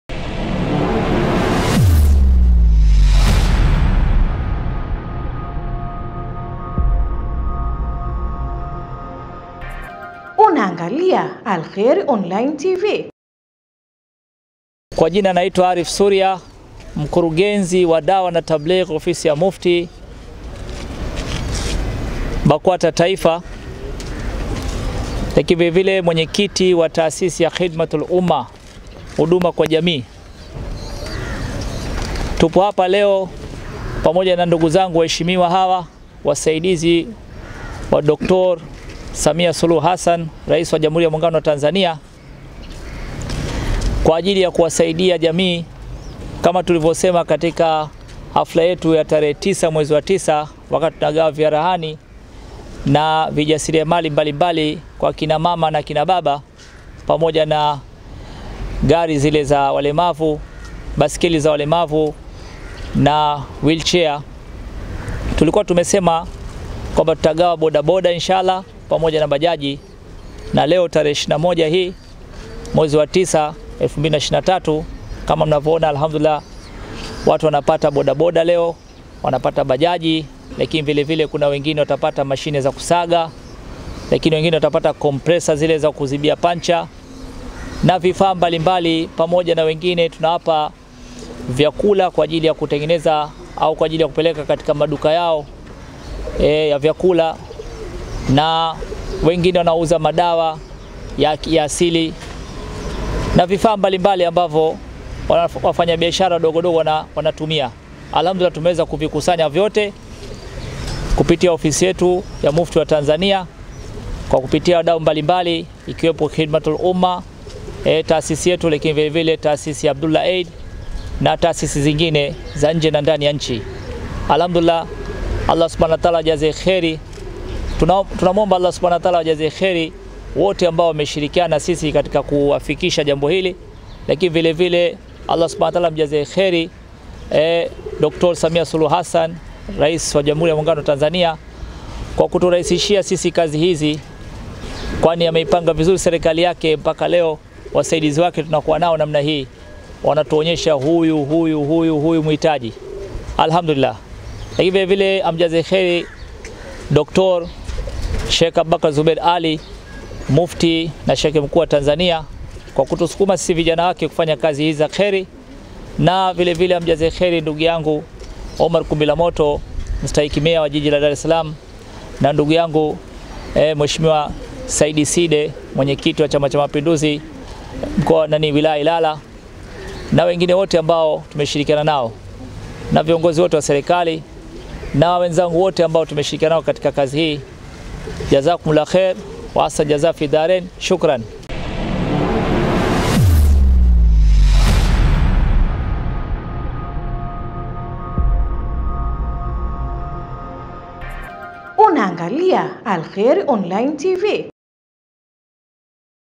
MEYA OMARY KUMBILAMOTO AUNGANA NA MKURUGENZI WA TABLIGHI AARIFU KUWASAIDIA WAJASILIAMALI HAPA NCHINI
AKIZUNGUMZA KTK UGAWAJI WA VIFAA MBALI MBALI SHEIKH AARIFU AMELEZEA VITU WALIVYOVIGAWA KWA WANANCHI